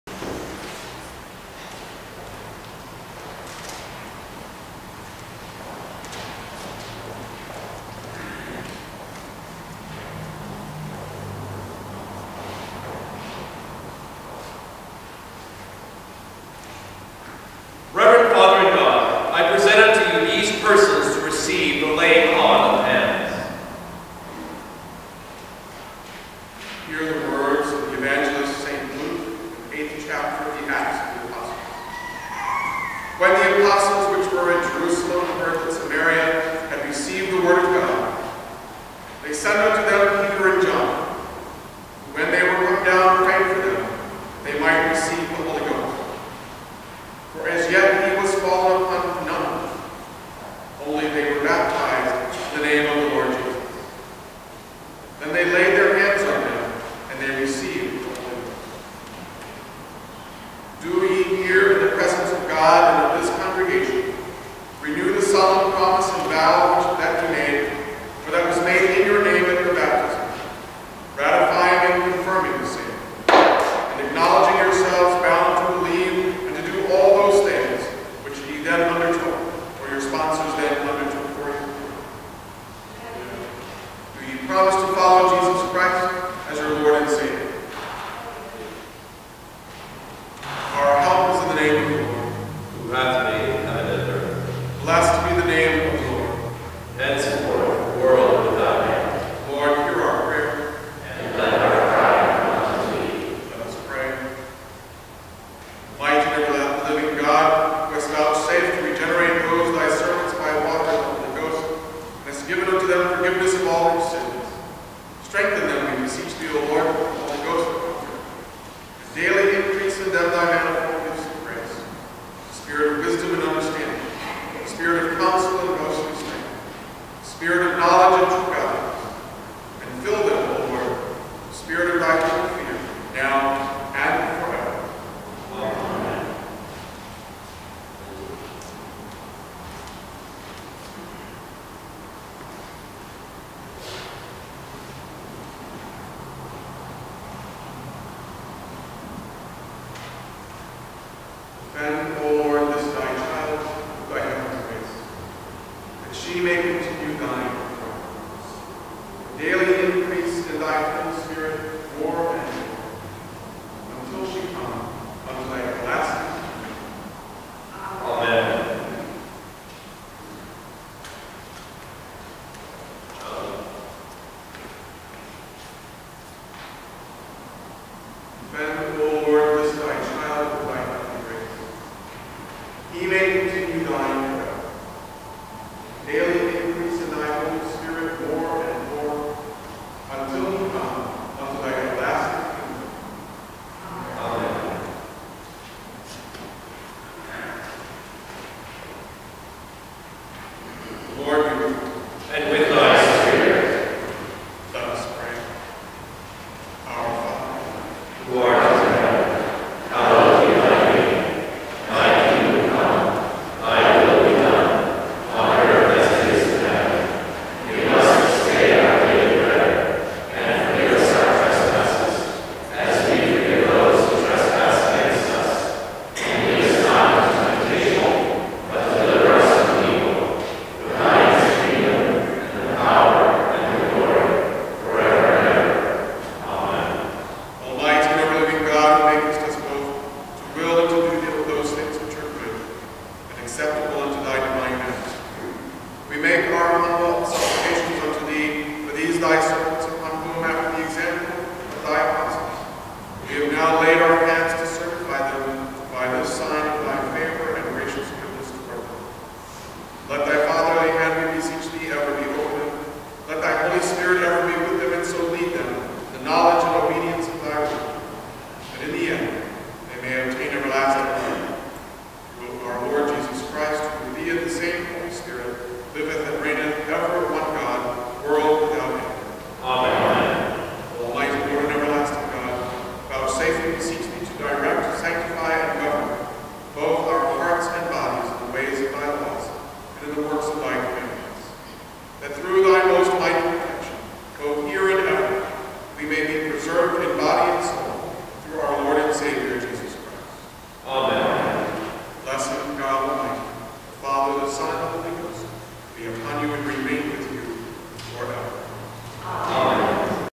The Order for Confirmation (1928 Book of Common Prayer) @ St. John's Detroit
John and Lindsey Gardner were Confirmed in the faith, Lent 1, 2015, by The Rt. Rev'd Wendell N. Gibbs, Jr., Diocese of Michigan.